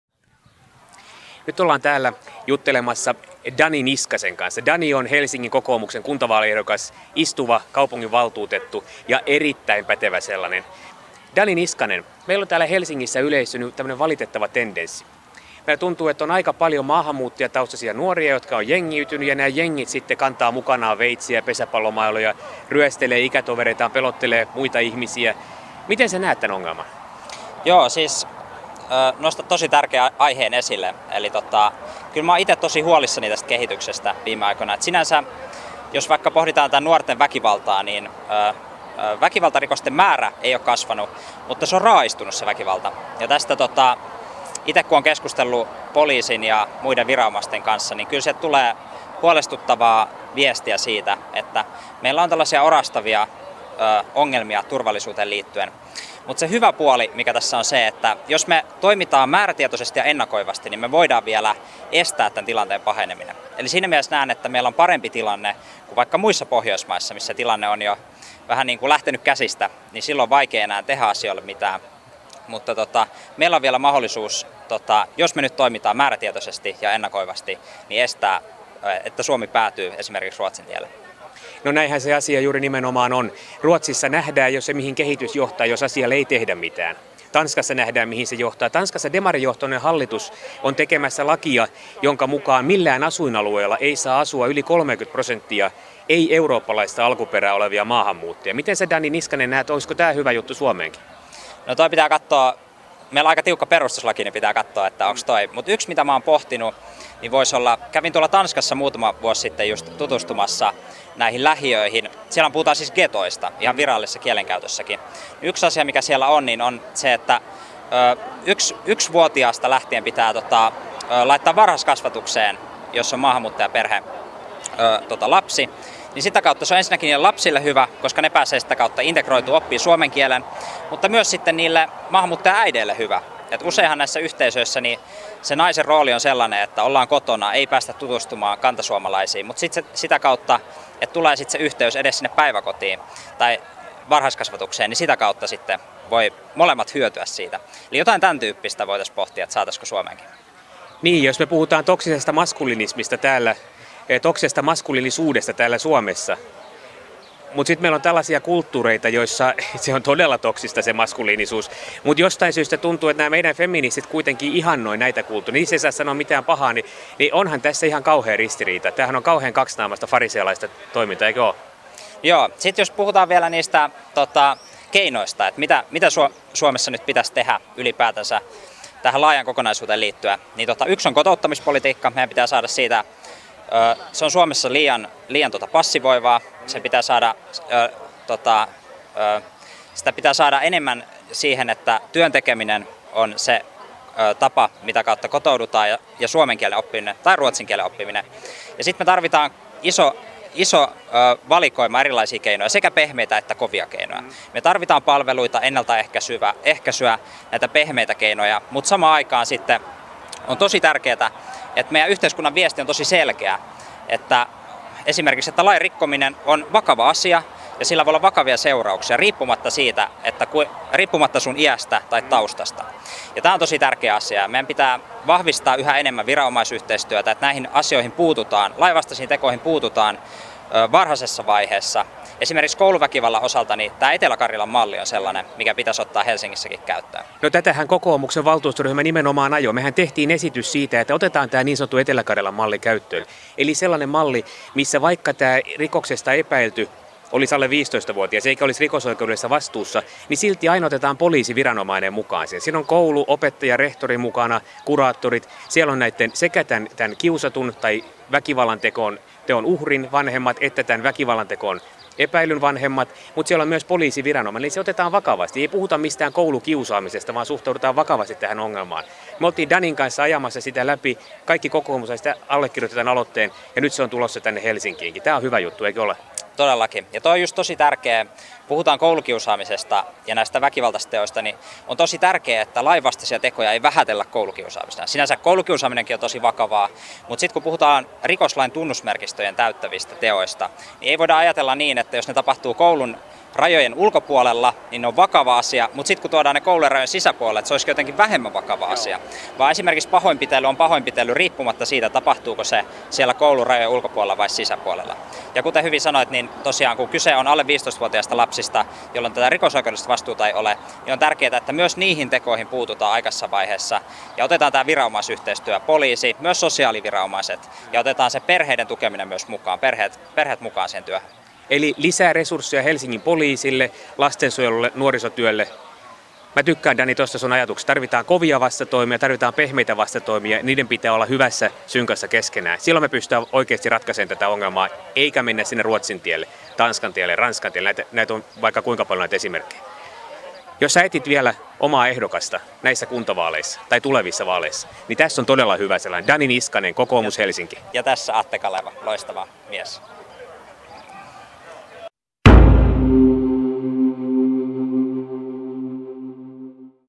Dani Niskanen ja Atte Kaleva: turvallinen Helsinki
Keskustelimme ystäväni ja valtuustokollegani Dani Niskasen kanssa turvallisuudesta, maahanmuutosta, kotouttamisesta, nuorten jengiytymisestä ja yleensä siitä, miten vältämme Ruotsin virheet. Vielä ei ole liian myöhäistä, mutta aikaa ei ole hukattavaksi.

Nyt on sinun vuorosi vaikuttaa. Sinun äänelläsi on merkitystä. Muista äänestää turvallisen kaupungin puolesta.

Dani Niskanen on ehdolla kuntavaaleissa, kuten minäkin. Helsingin Kokoomus